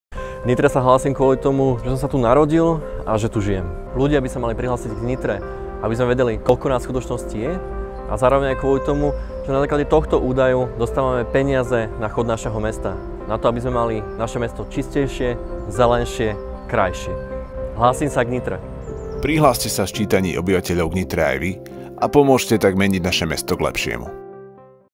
Hlásim sa k Nitre - Marek Hattas
Prihláste sa v sčítaní obyvateľov k Nitre aj vy a pomôžte meniť naše mesto k lepšiemu. Elektronické sčítanie obyvateľov prebieha od 15. februára do 31. marca 2021.